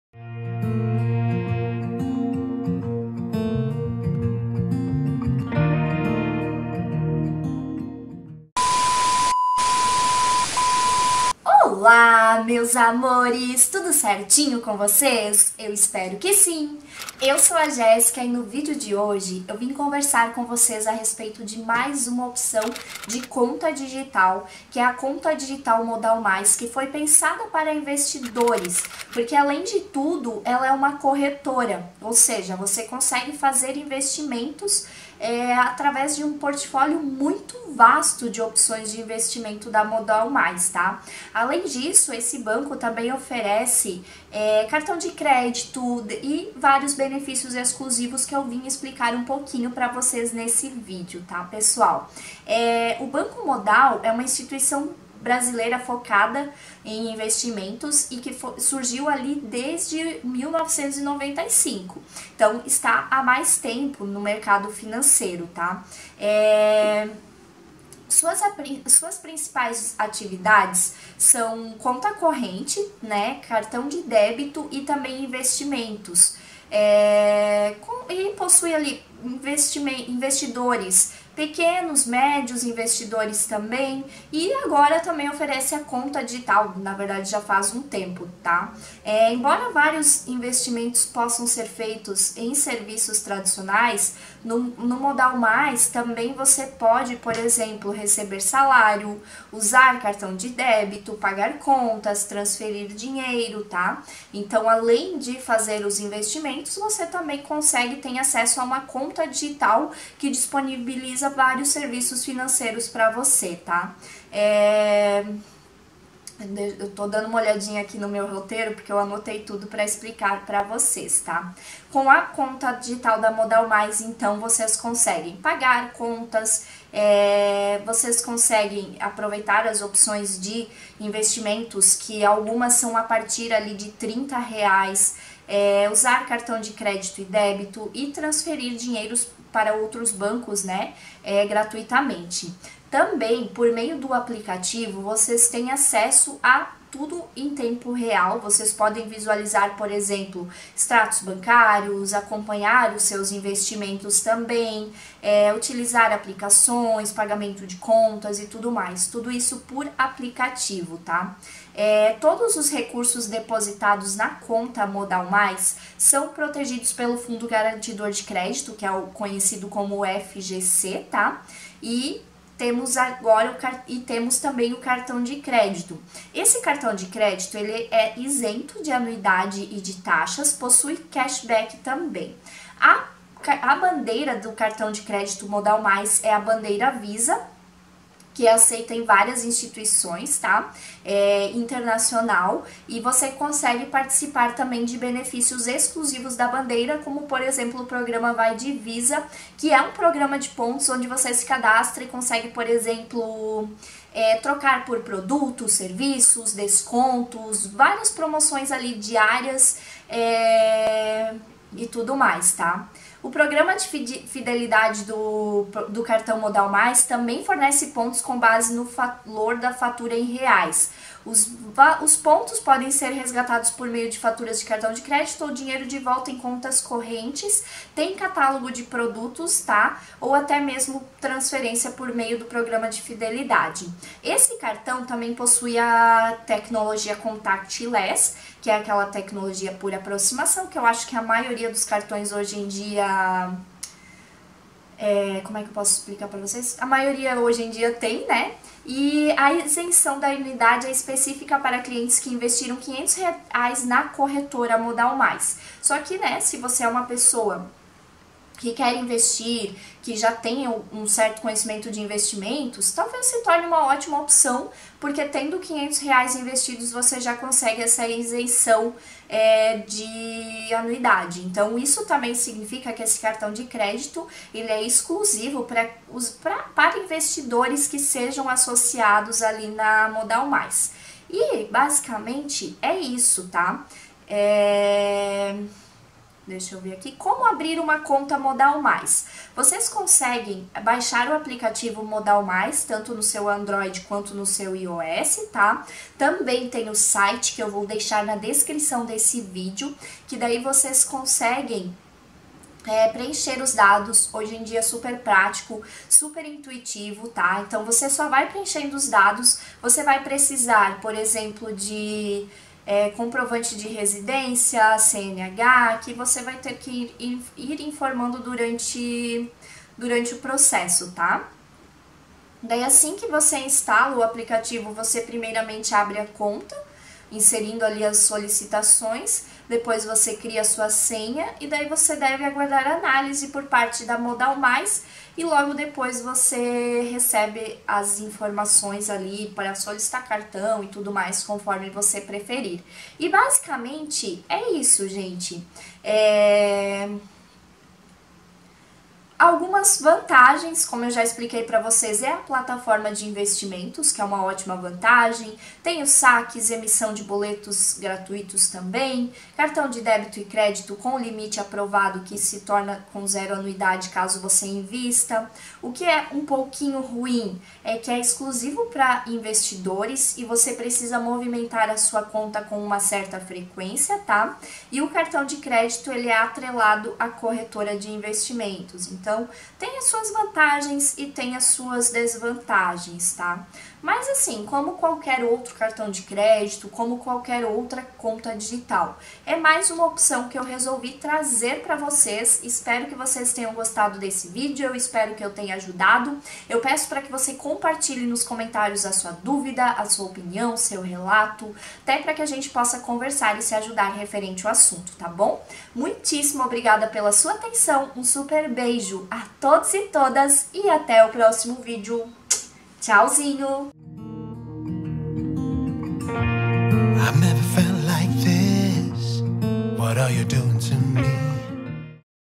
MODALMAIS | CONTA DIGITAL para INVESTIDORES | Vale a Pena?
No vídeo de hoje eu vim falar de uma conta digital completa para investidores. Para quem não conhece a Modalmais é uma corretora de investimentos que trouxe também a sua conta digital para facilitar esse processo aos seus investidores. No vídeo eu explico um pouco mais sobre os benefícios e também funcionalidades oferecidas. Espero que esse vídeo ajude. Peço para que compartilhe sua dúvida, opinião ou relato para que possamos conversar e nos ajudar referente o assunto. Muitíssimo obrigada pela sua atenção, super beijo e até o próximo vídeo.